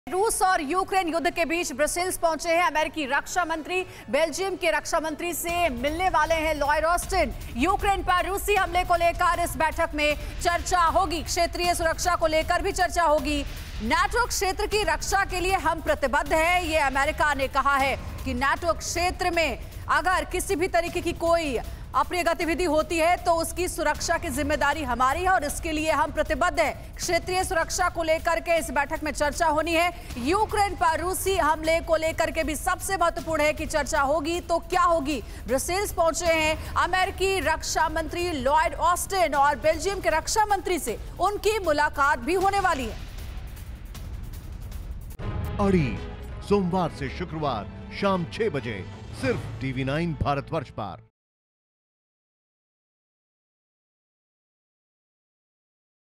War Zone : बेल्जियम के रक्षा मंत्री से मिलेंगे लॉयड ऑस्टिन, Ukraine पर रूसी हमले को लेकर होगी चर्चा
युद्ध के बीच ब्रुसेल्स पहुंचे अमेरिकी रक्षा मंत्री. बेल्जियम के रक्षा मंत्री से मिलेंगे लॉयड ऑस्टिन  और यूक्रेन पर रूसी हमले को लेकर होगी चर्चा ।

Credit